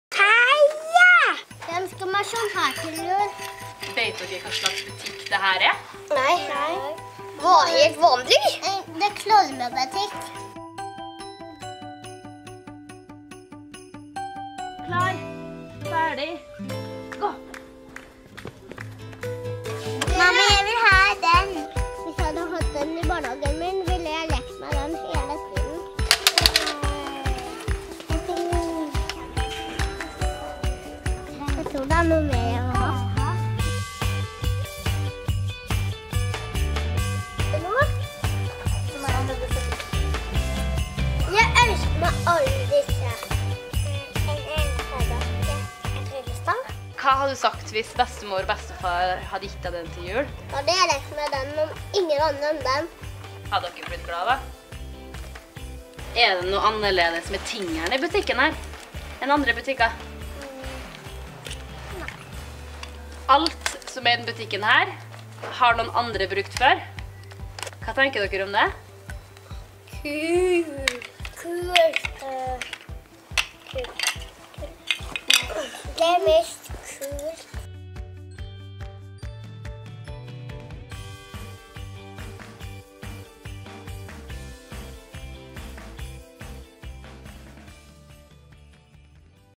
Hva tenker barn om å få brukte julegaver?
Vi lurte på hva barn (egentlig) tenker om å få brukte gaver til jul. Så vi tok de med til M&E Second Hand for å finne det ut.